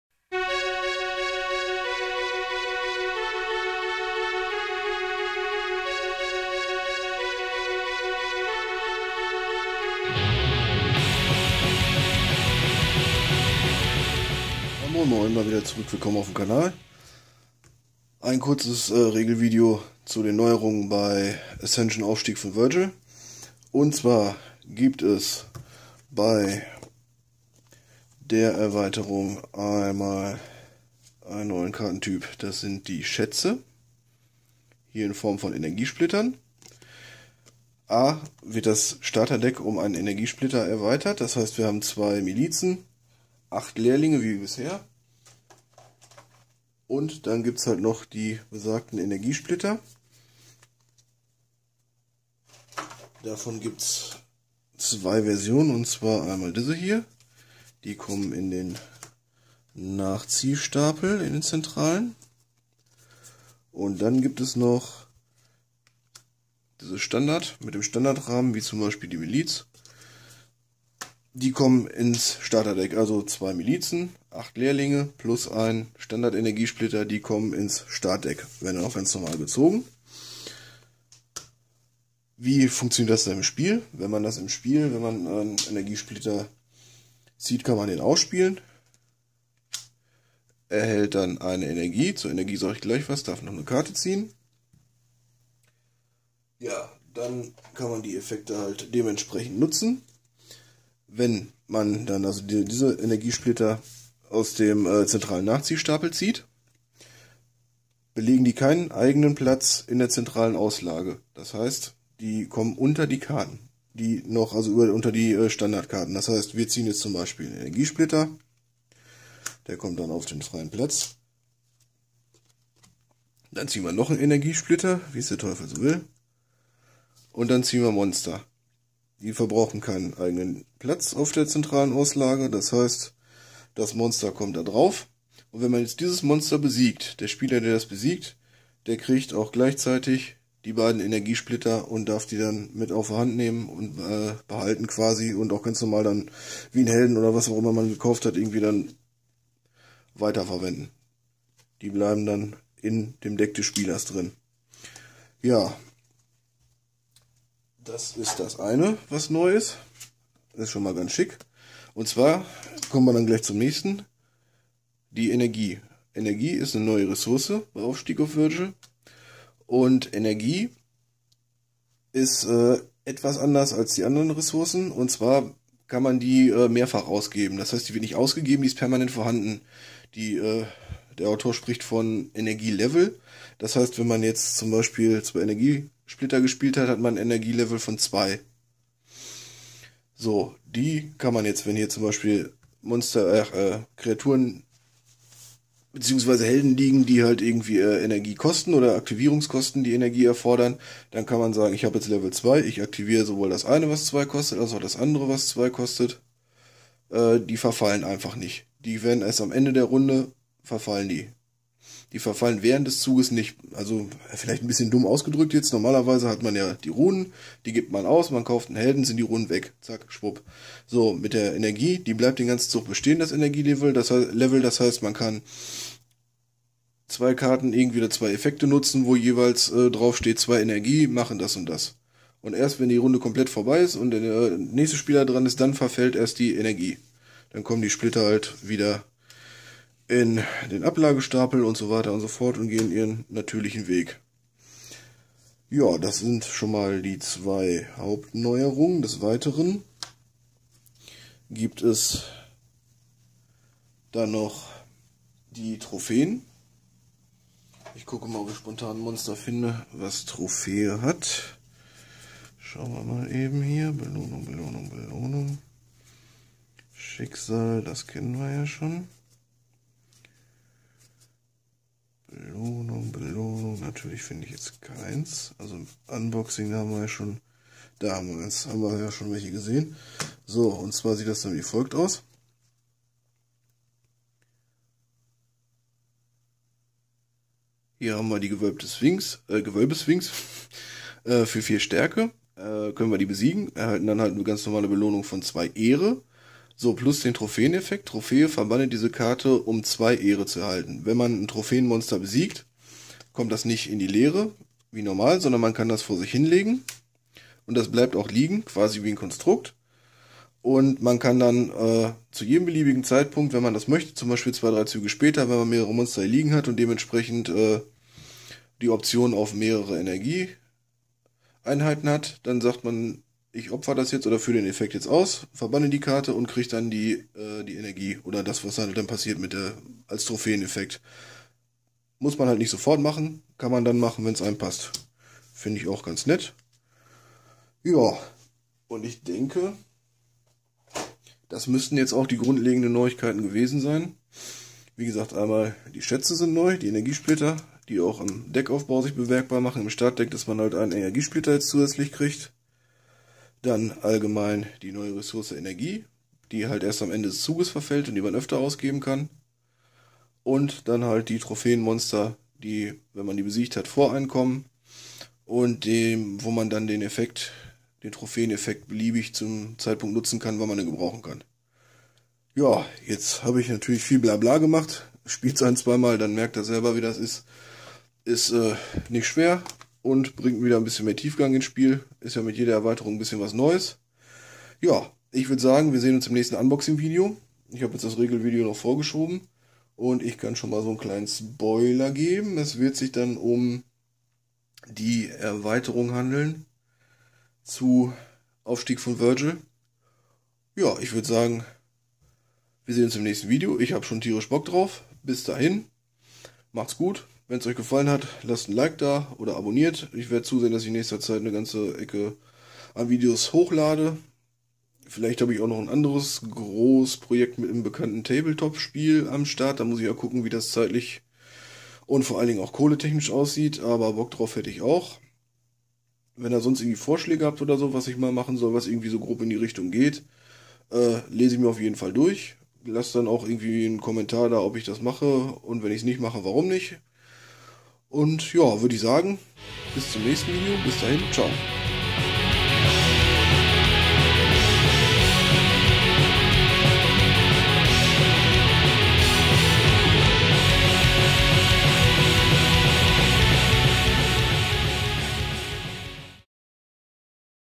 Ascension - Aufstieg von Vigil Neue Regeln
Hier eine kurze Zusammenfassung der bei Aufstieg von Vigil eingeführten neuen Regeln (Energiesplitter & Energie). Viel Spaß damit :-)